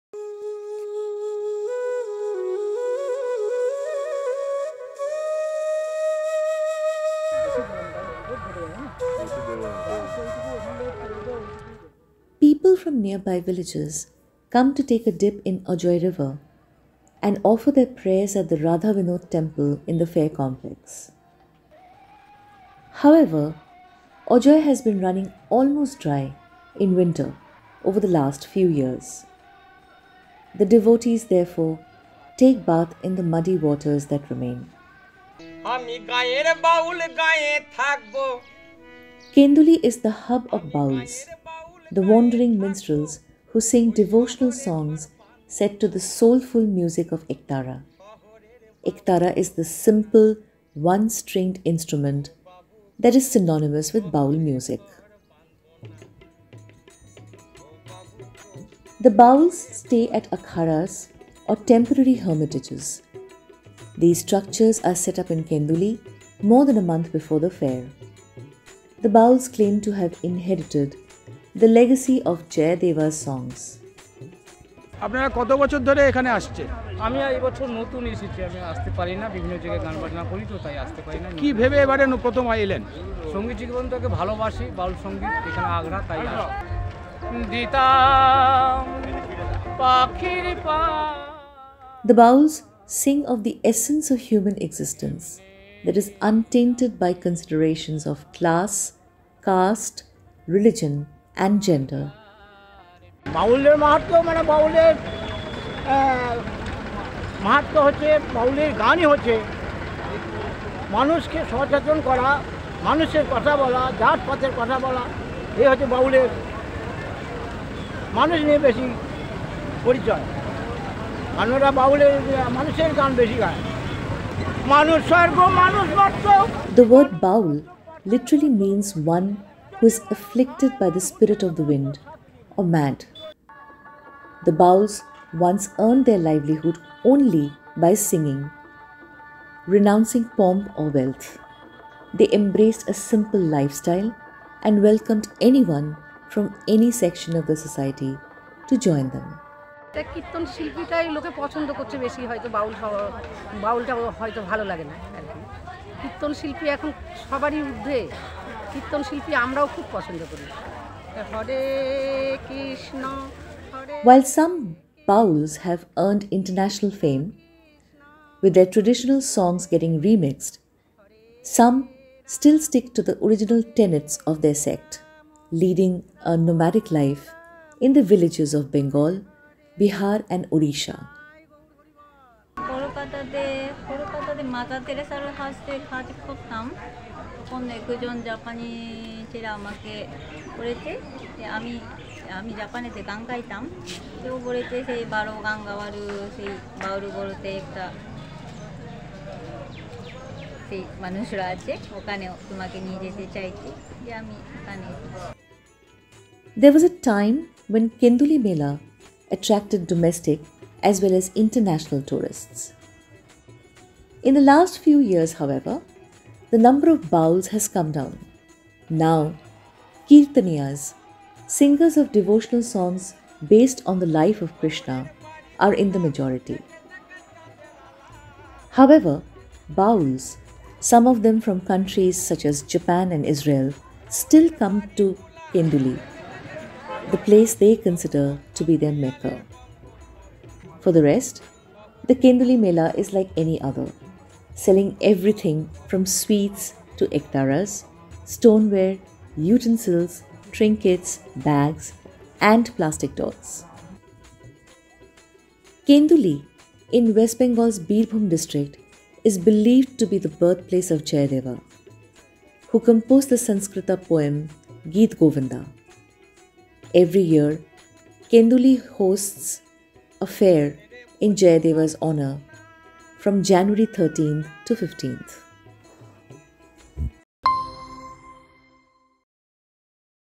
West Bengal’s Kenduli comes alive during the annual Baul Mela
Kenduli in West Bengal’s Birbhum district is believed to be the birthplace of Jayadeva, who composed the Sanskrit poem “Gita Govinda”. Every year, a fair is organised in Kenduli in Jayadeva’s honour from January 13 to January 15.

Credits:
Script: Anusua Mukherjee
Voice: Aditi Sengupta
Visuals: Ashoke N Dey
Editing: Razal Pareed
Produced by: Jinoy Jose P.